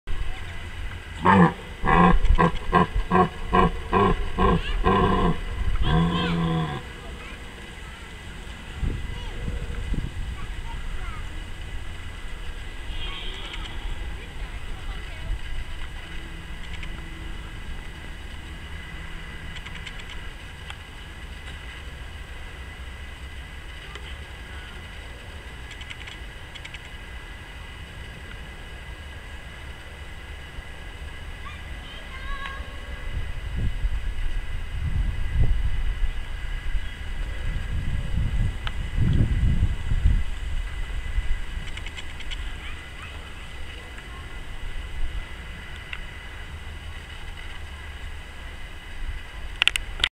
Stag Grunts and Struts, Bedfords Park, Havering Atte Bower, Greater London
This stag roars to size up to other stags to compete for the right to mate with the hinds

Video taken on Friday 7 October 2011